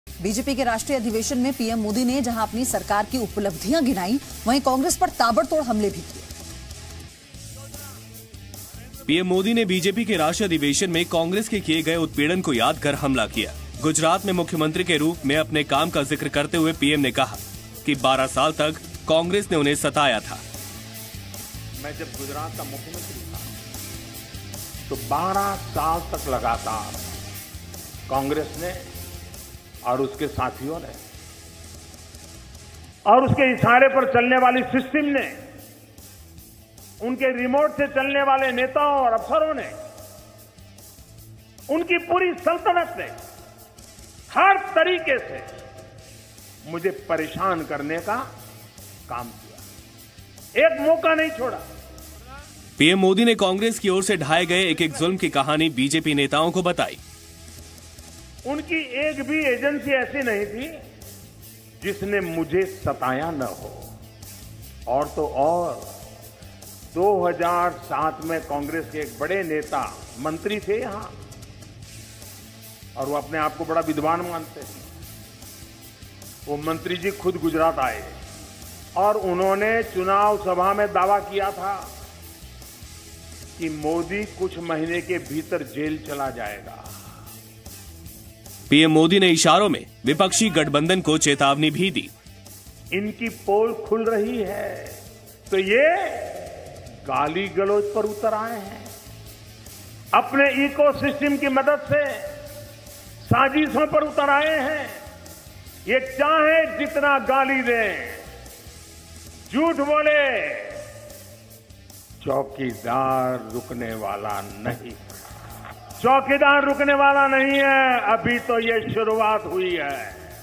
Morning Breaking: Modi attacks Congress in BJP National Council Meet
While addressing party workers in BJP  National Council Meet, PM Narendra Modi slammed Congress. To know more watch the report.